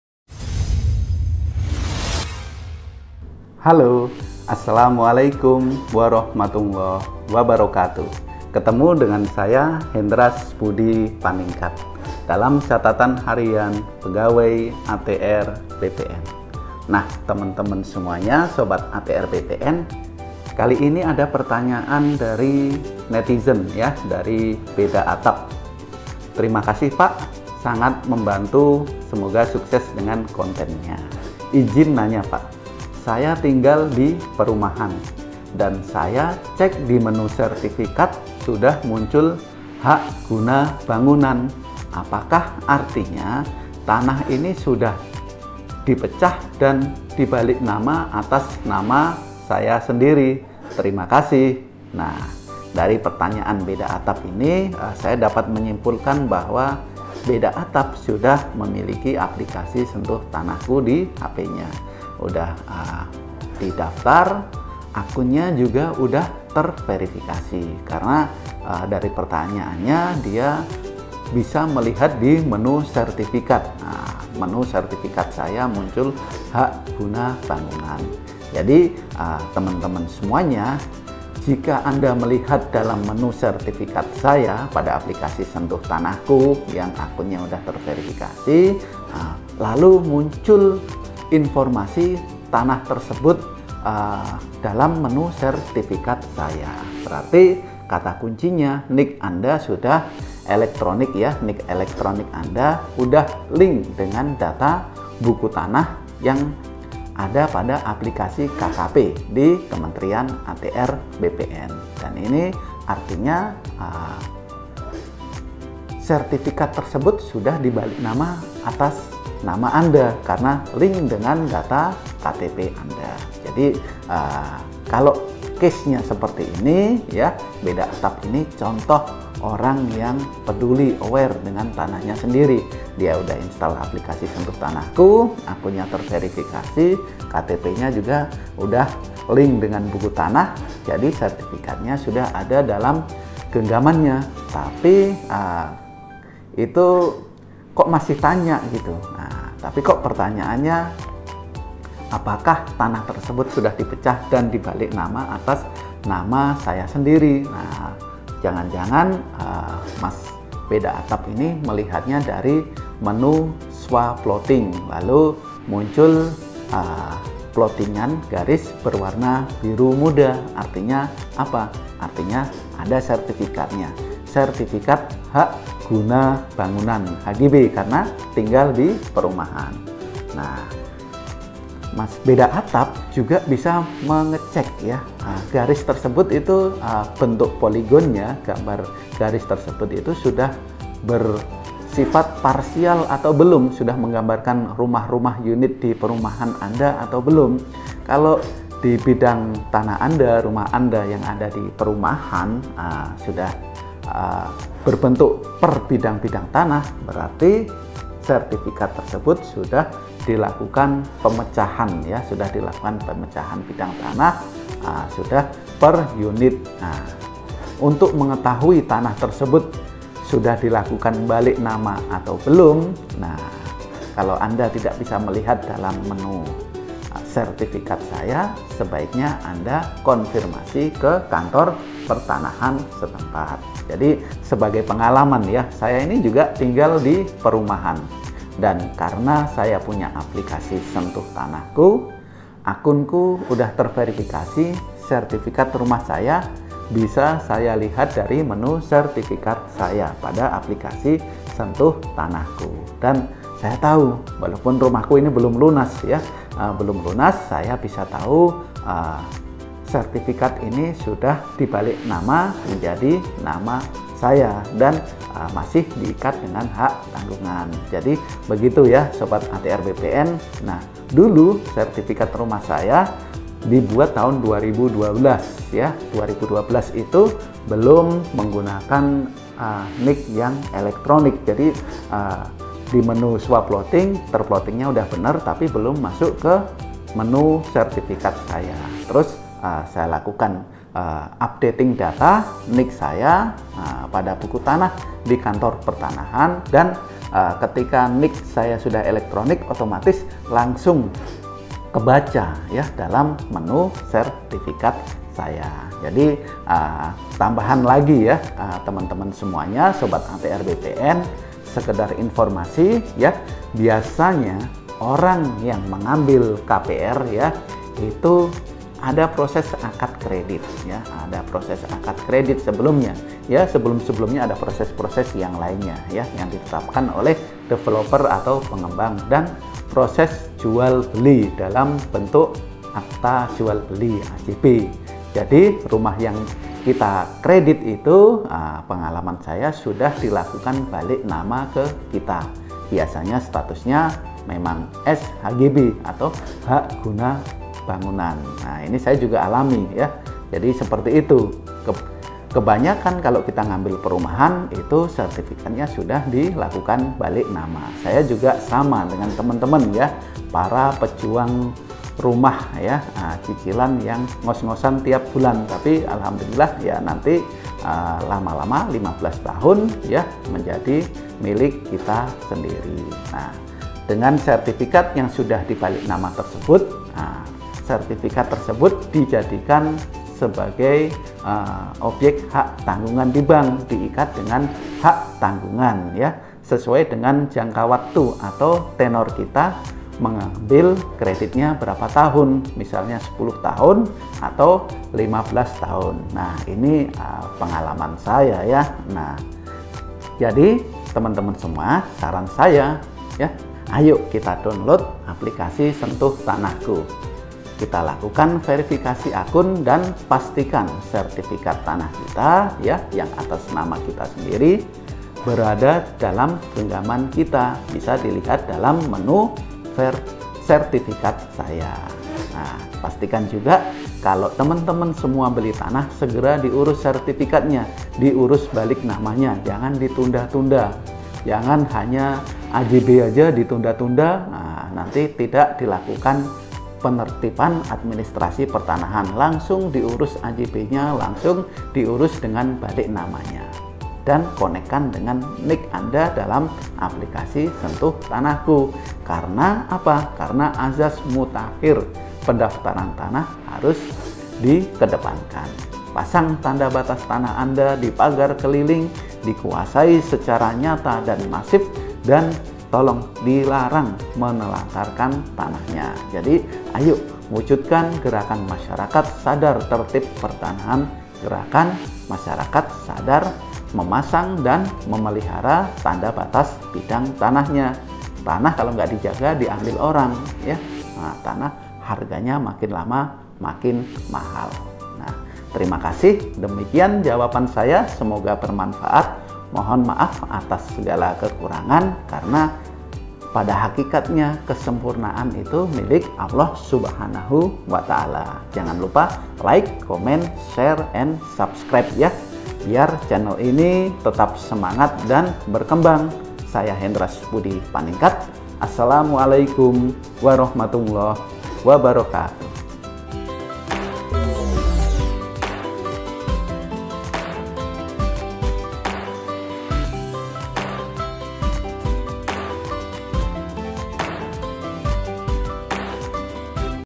Cara Mudah Cek Sertipikat Perumahan Dengan Aplikasi Sentuh Tanahku
Video ini menjelaskan tentang tanya jawab seputar pertanahan bagaimana cara mengecek tanah perumahan sertipikat hak guna bangunan sudah dipecah atau belum dan sudah di balik nama atau belum secara mudah dan cepat menggunakan aplikasi sentuh tanahku